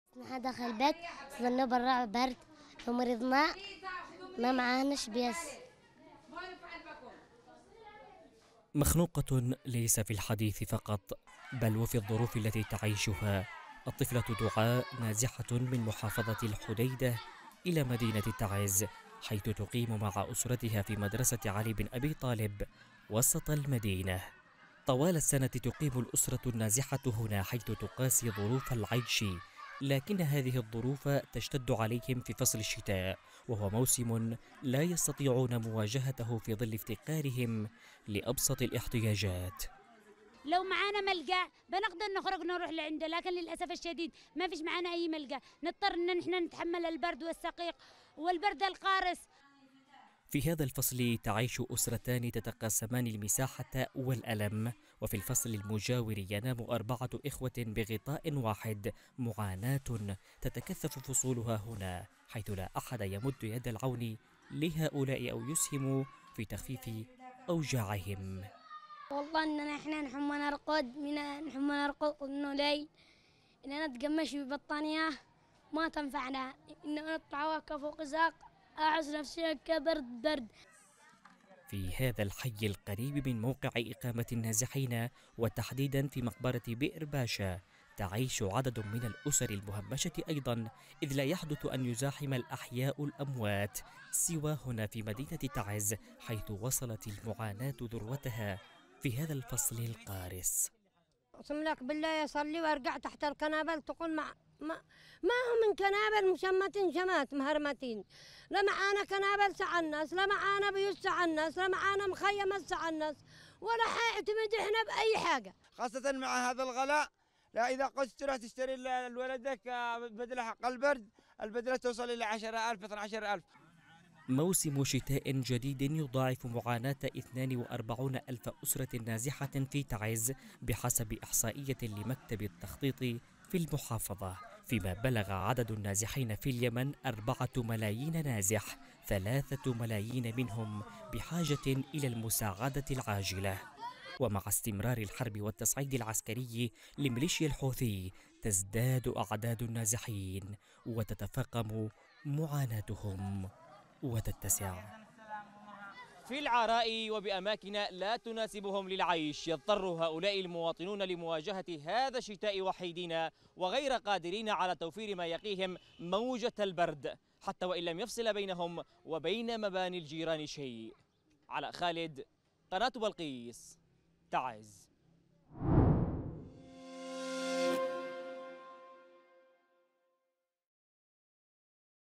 محافظة تعز.. كاميرا بلقيس توثق معاناة النازحين والمهمشين مع دخول فصل الشتاء | تقرير: علاء خالد
يعيش النازحون في المخيمات أوضاعا معيشية صعبة، تضاعفت مع دخول فصل الشتاء .
وتتفاقم معاناة النازحين والمهمشين في محافظة تعز مع وصول موجات الصقيع إلى ذروتها في وقت لا يستطيعون فيه توفير ما يقيهم وطأة عوامل الطقس القاسية .